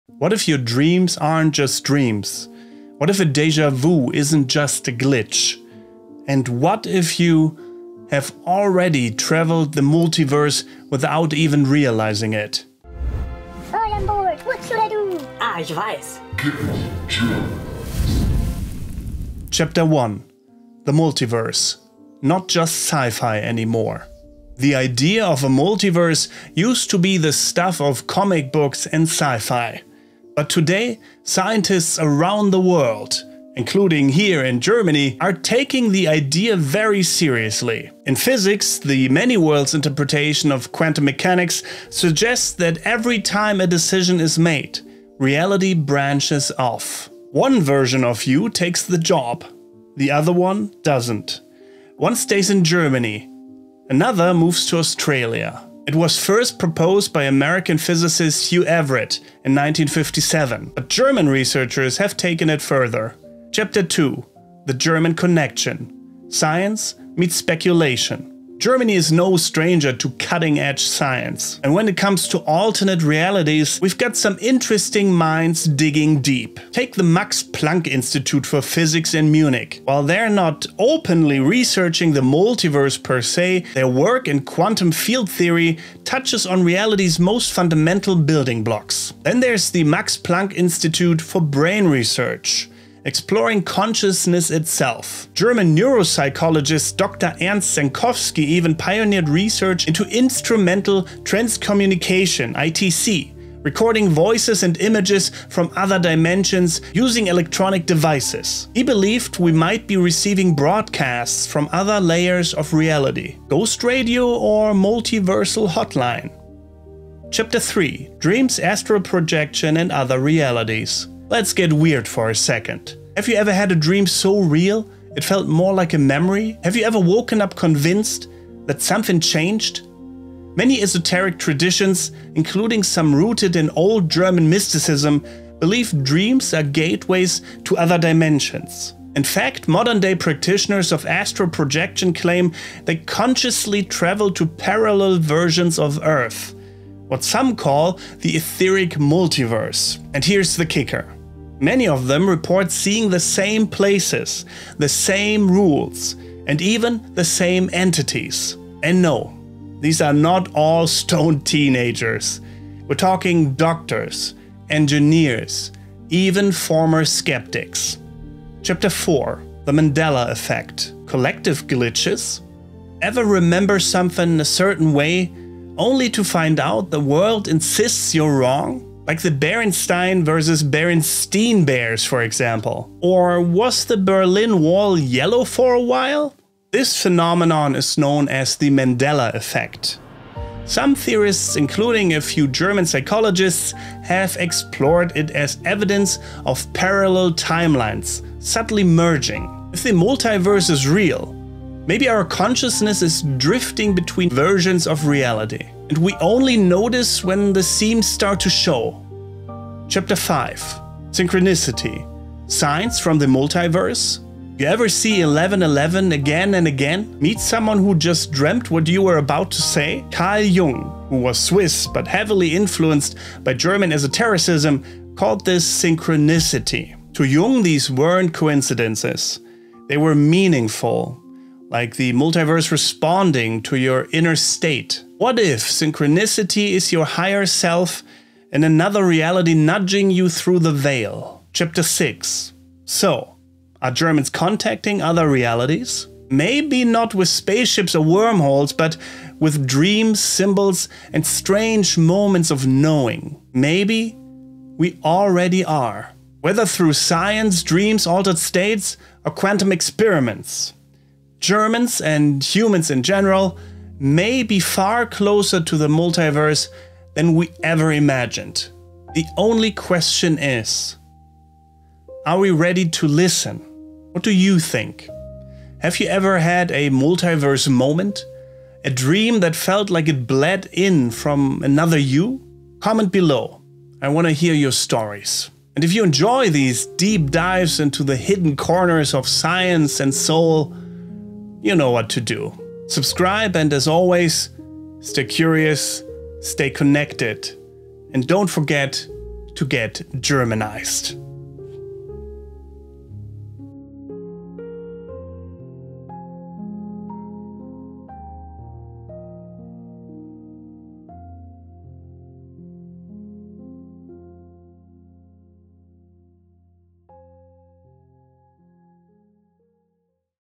Are Germans Contacting Other Realities? | Multiverse, Dreams, and Synchronicity Explained
Are we living just one life... or infinite versions of it across countless realities? And what if Germans, through science, dreams, and synchronicity, are already in contact with those other worlds?

In this video, we dive into the Multiverse Theory, dream states, astral projection, the Mandela Effect, and the role of German thinkers, scientists, and mystics in decoding it all.

From quantum physics to Jungian psychology, we explore how parallel timelines, subconscious travel, and strange synchronicities could be more than just imagination, they might be the whispers of other worlds.

🌌 Topics covered:

The Many Worlds Interpretation of quantum mechanics

Work by German researchers like Dr. Ernst Senkowski

Dreams and astral projection as interdimensional experiences

The Mandela Effect and timeline anomalies

Carl Jung, synchronicity, and archetypal connections

👇 Share your own multiverse experiences or strange synchronicities in the comments!

📚 Bonus visuals + companion PDF for patrons and channel members.

for more videos and information about Get Germanized, Germany and the German language!